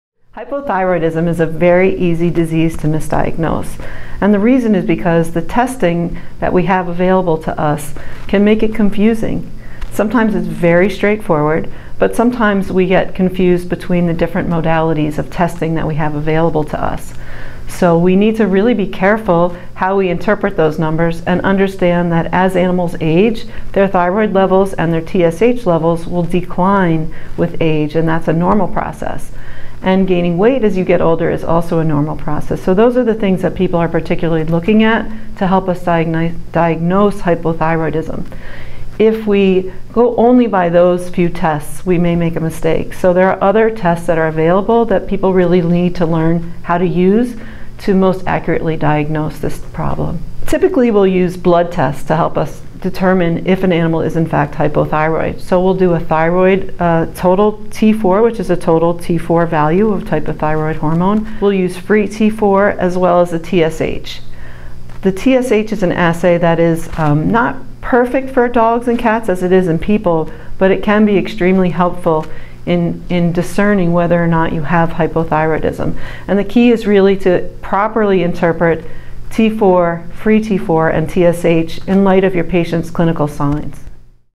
Why is Hypothyroidism Easily Misdiagnosed?
Diane Levitan, VMD, DACVIM, from Peace Love Pets Veterinary Care, explains why hypothyroidism is so easily misdiagnosed.